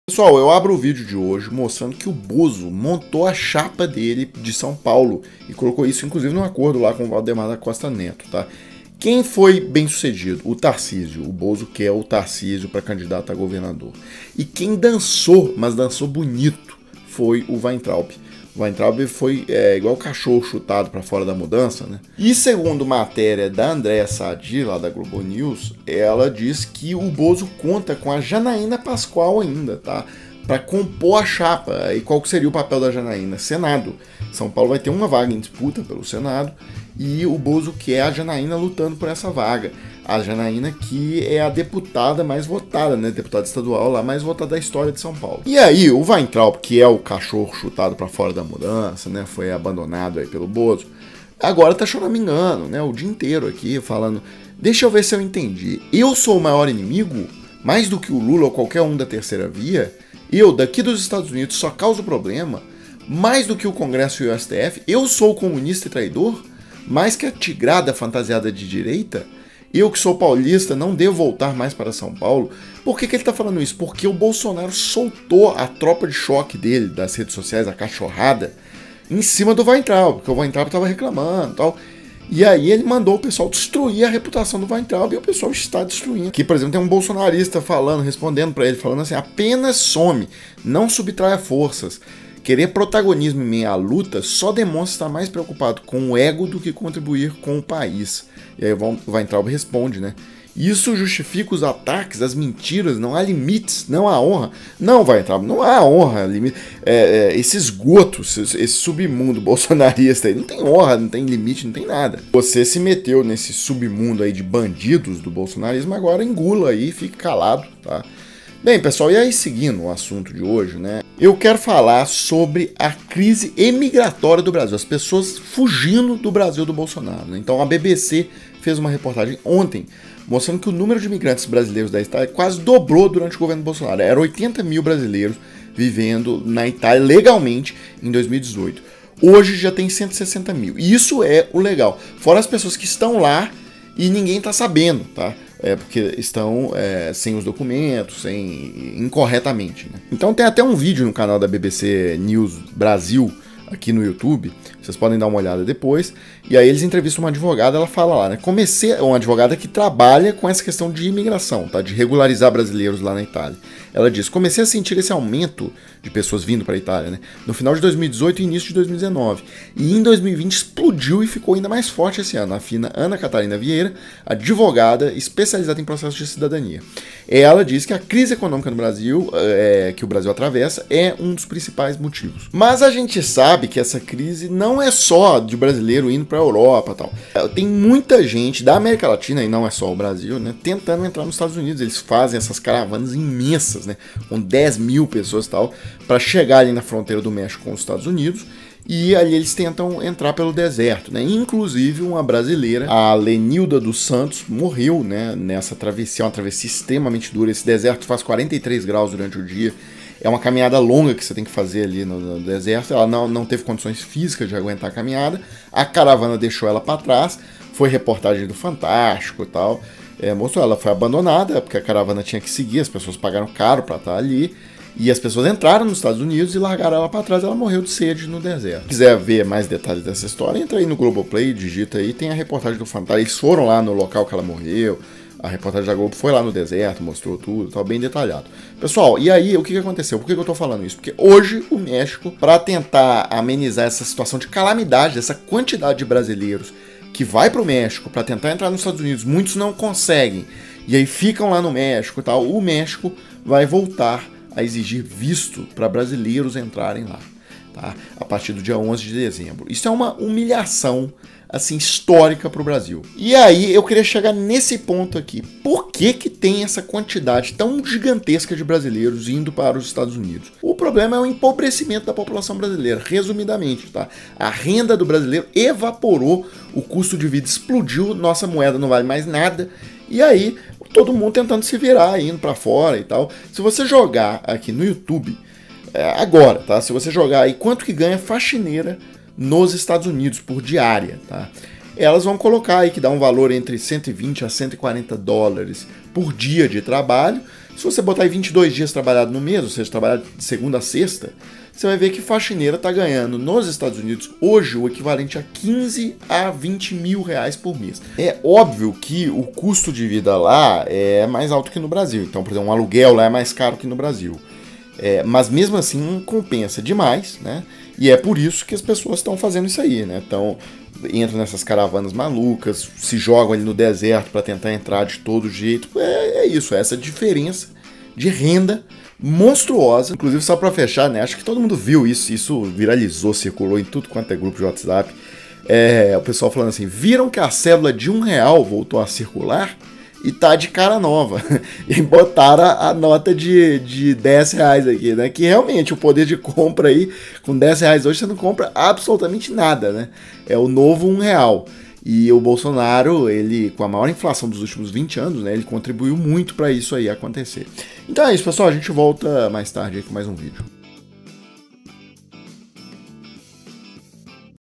Bozo escala "equipe" para 202 com Janaína e Tarcíso
BOZO ESCALA EQUIPE P ELEIÇÃO 22 COM JANAÍNA E TARCÍSIO, CHUTANDO WEINTRAUB, QUE CHORAMING